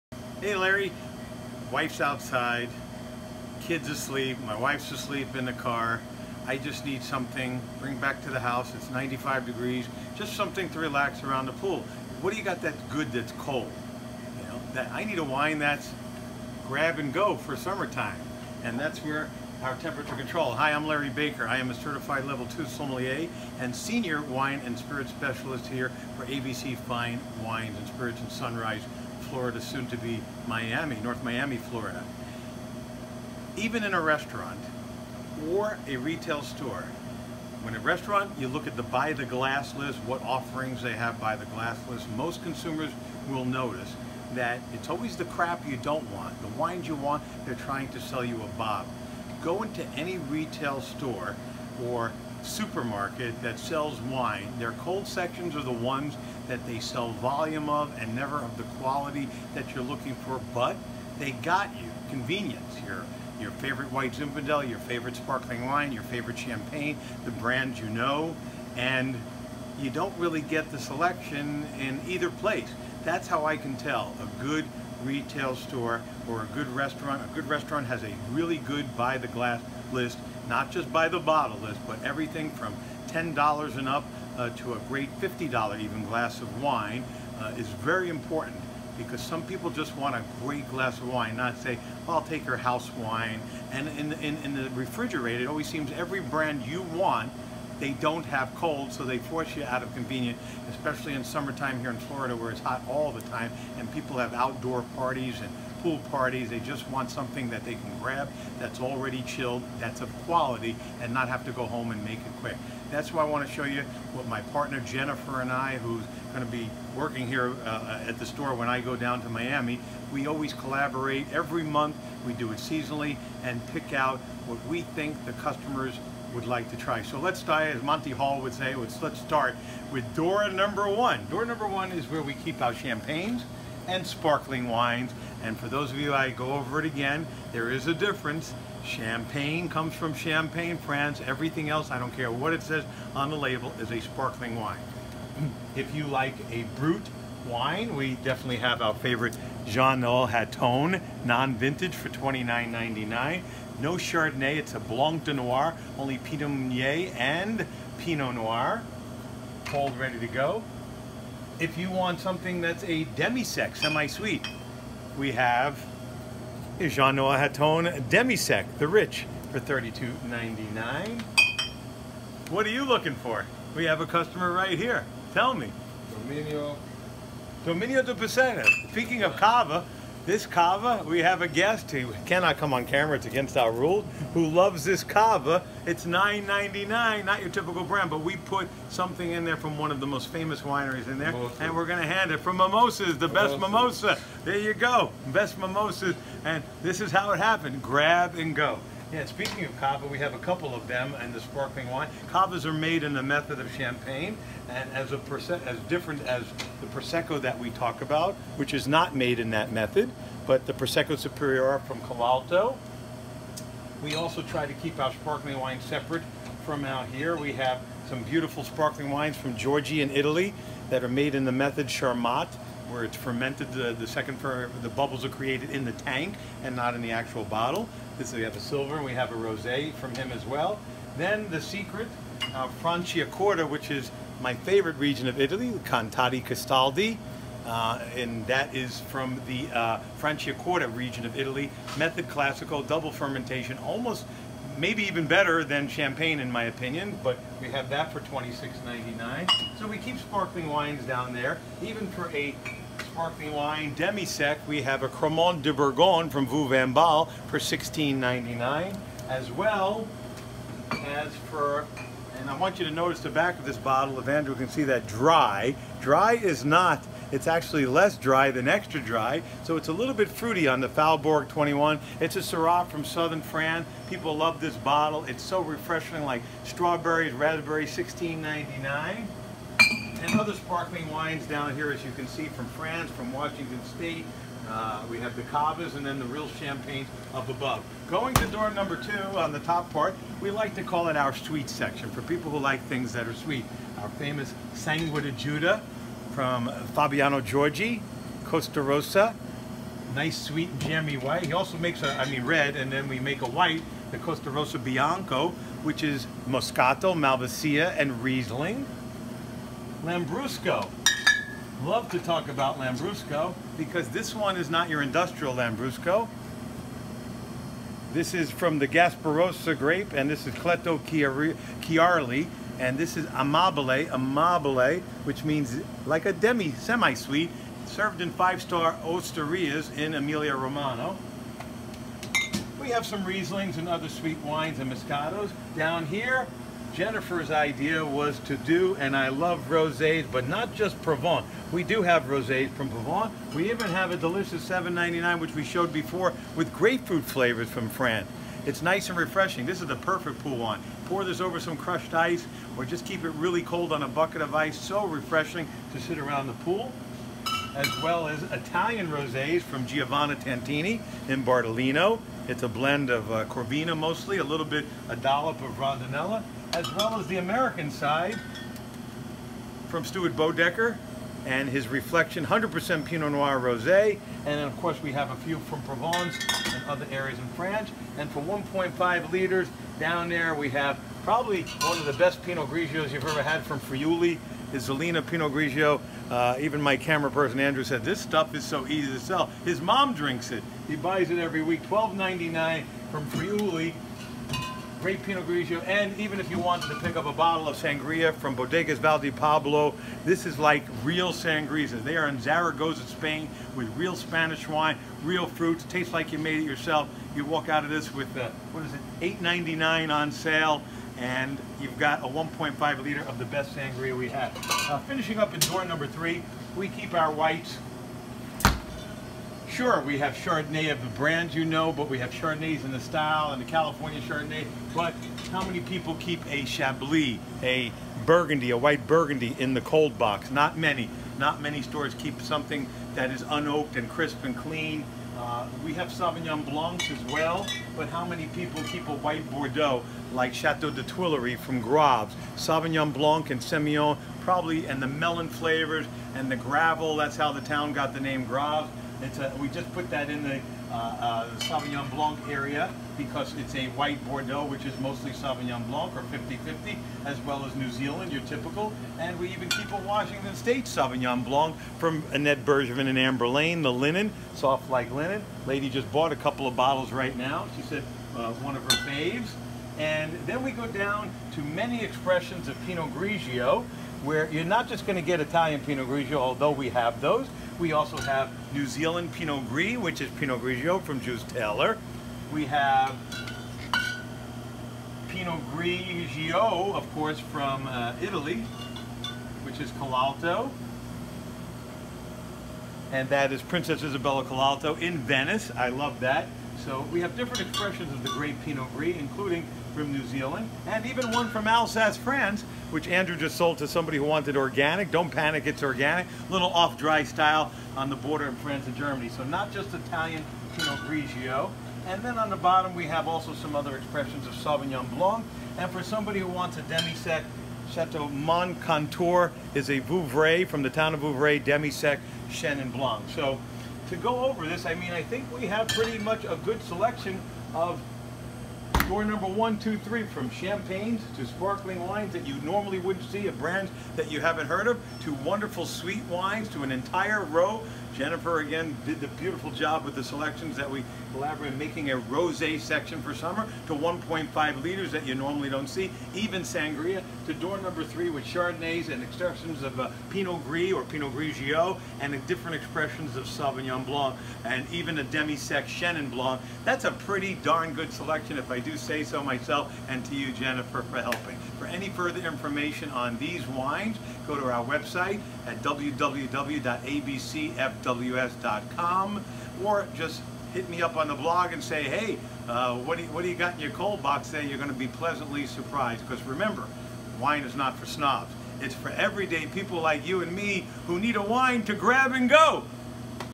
Chilled Wines To “Grab And Go”
Certified Level 2 Sommelier And Senior Wine And Spirits Specialist For ABC Fine Wine And Spirits In South Florida, Larry Baker, Discusses How Important It Is That Any Wine Retailer Have A Diversified Selection Of Chilled Wines For Grab And Go